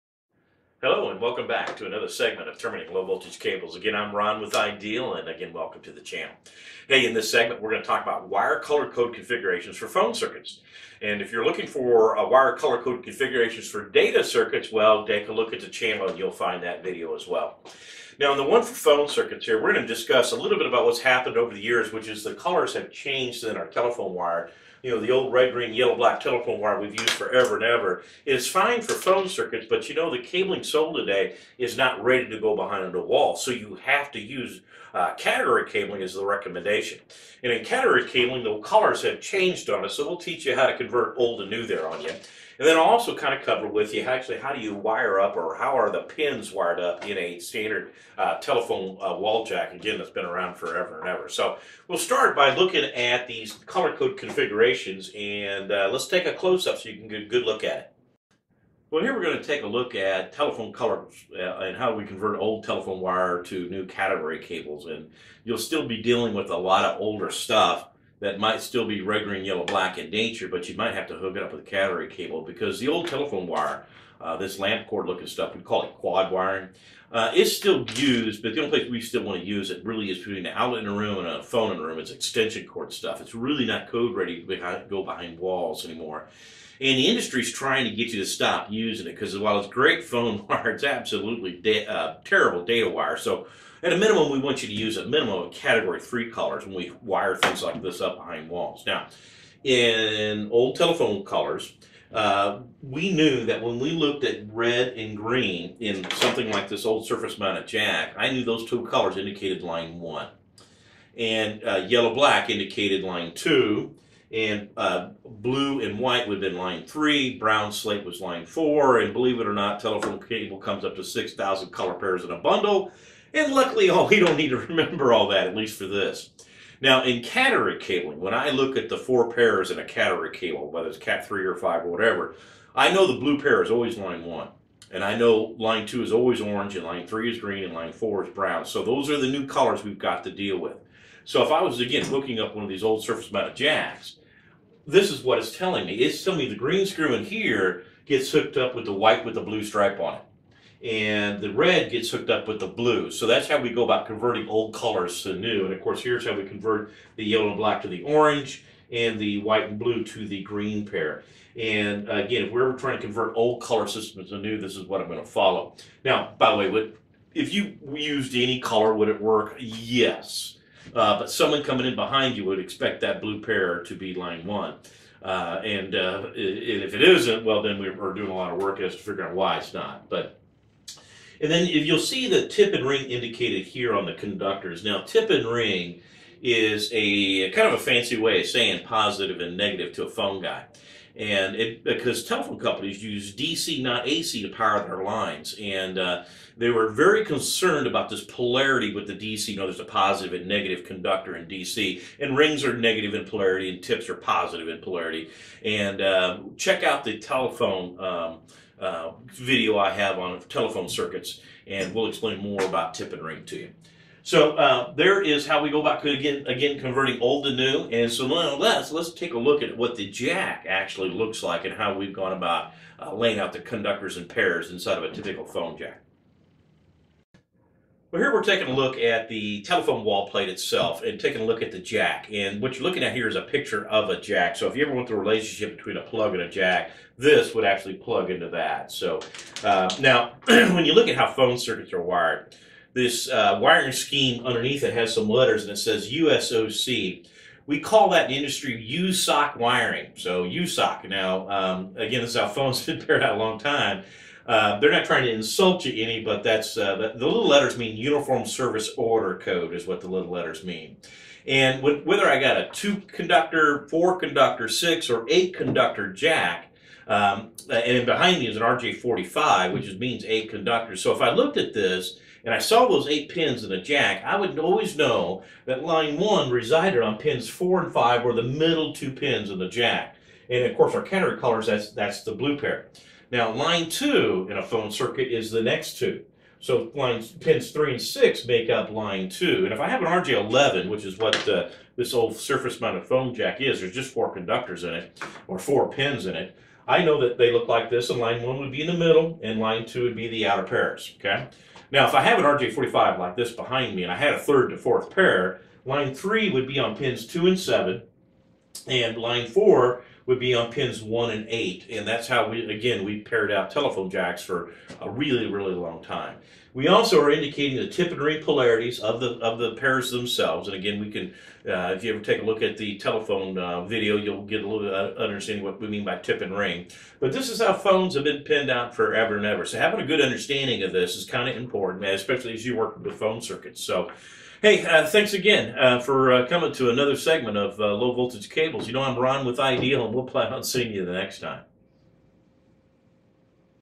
Wire Color Code Configuration for Phone
Ron discusses wiring for phone circuits and USOC wiring. He covers the pair colors and their positions in the jack and how to convert old wire colors to the new Cat 5e colors.